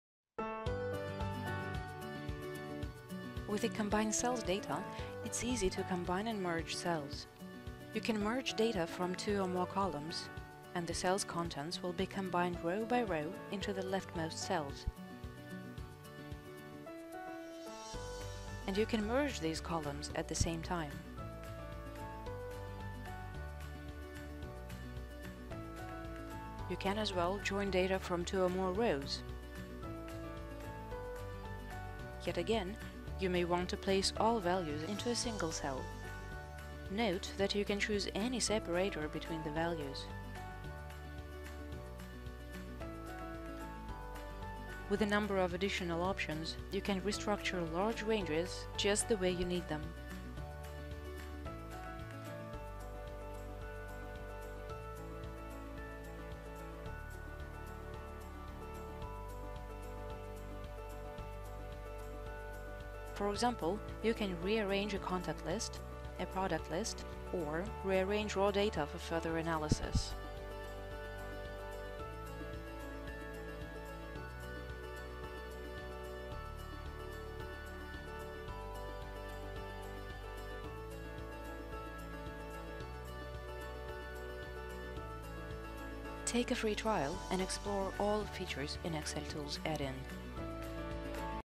Easily combine cells contents and merge cells in Excel without losing data
This brief overview of the XLTools Combine Cells Data for Excel shows:

- How to join, combine and merge cells without losing data
- How to combine rows, columns, or place data into a single cell
- How to combine cells contents and set any separator between the values

Suitable for Microsoft Excel 2016, 2013, 2010, 2007, and desktop version of Office 365 (32-bit and 64-bit).